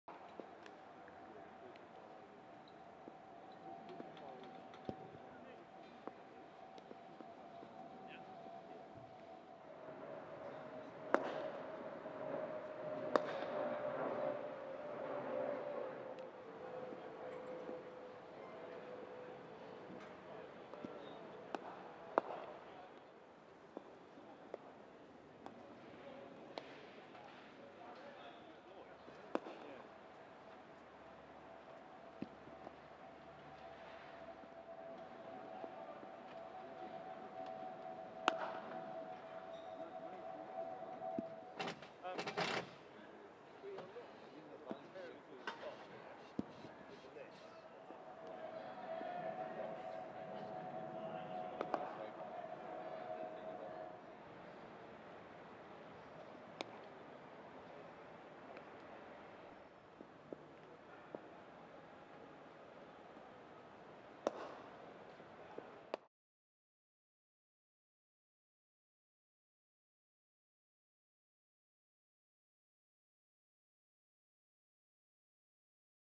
James Anderson and Chris Woakes bowl serious pace in the nets
England pace duo James Anderson and Chris Woakes bowl at good pace during England's net session while Moeen Ali sends down his spin deliveries at the Kia .